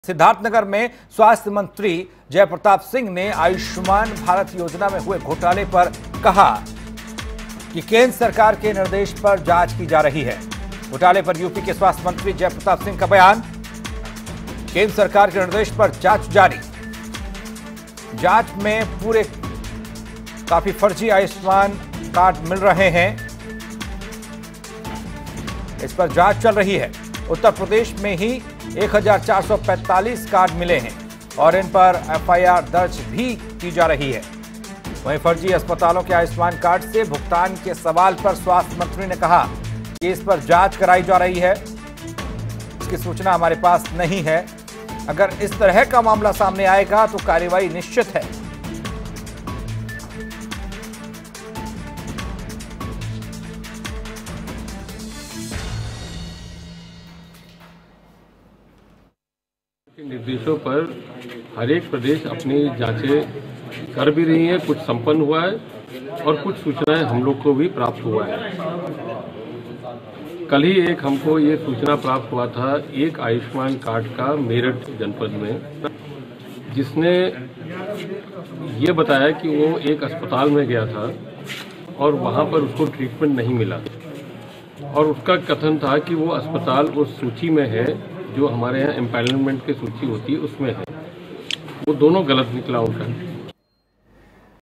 स्वास्थ्य मंत्री JaiPratapSinghका बयान, आयुष्मान भारत योजना में घोटाला
सिद्धार्थनगर में स्वास्थ्य मंत्री जय प्रताप सिंह ने आयुष्मान भारत योजना में हुए घोटाले पर कहा कि केंद्र सरकार के निर्देश पर जांच की जा रही है..